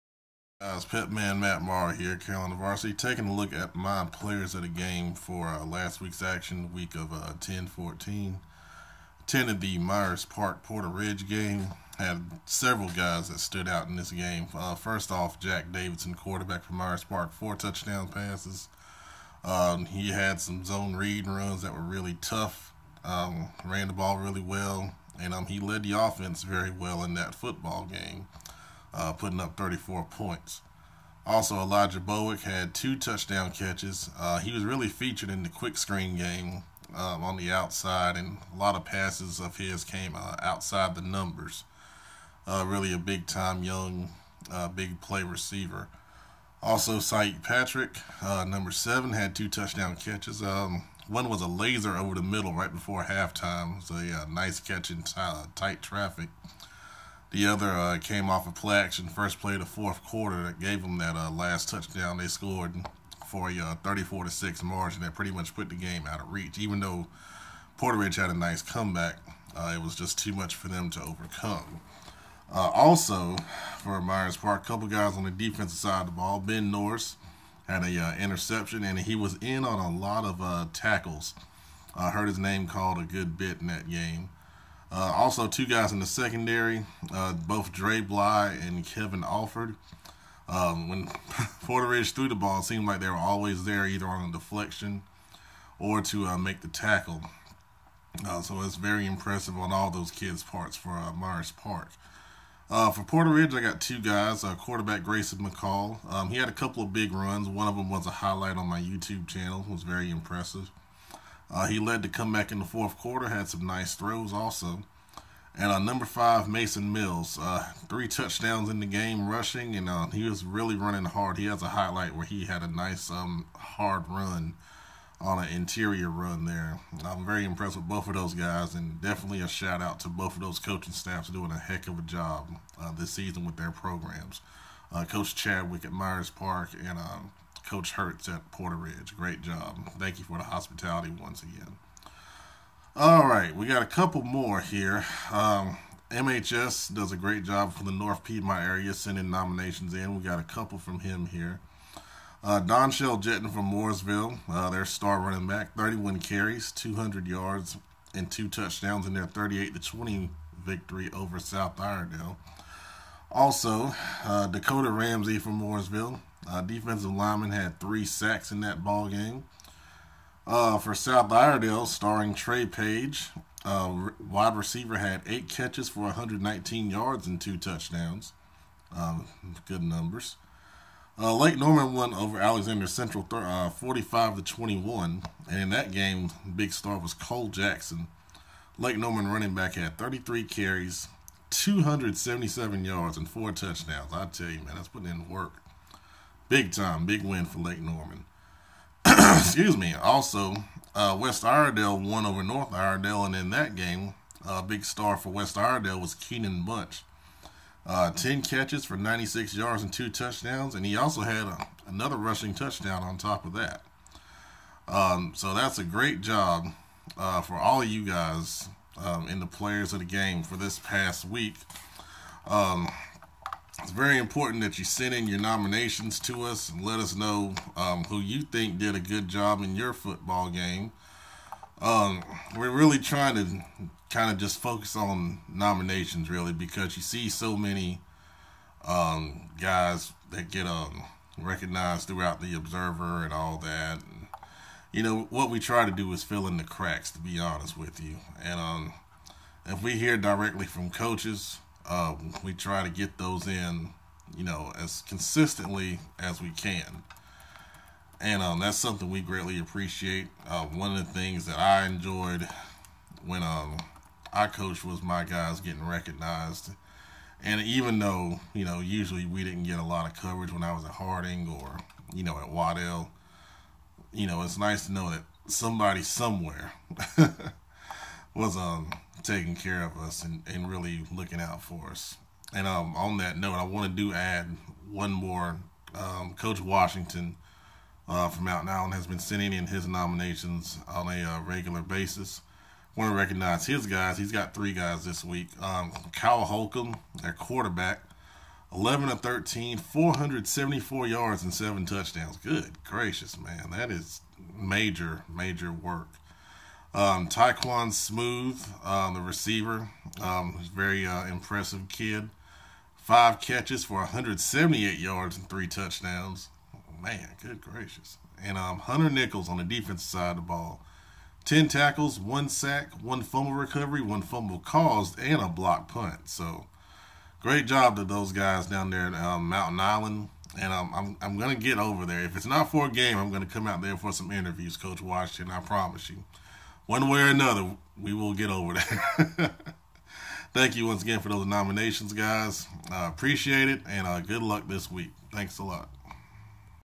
Players Of The Game - From Week Of 10/14
Players From Myers Park, Porter Ridge, Mooresville, South Iredell, Lake Norman, West Iredell, and Mountain Island Charter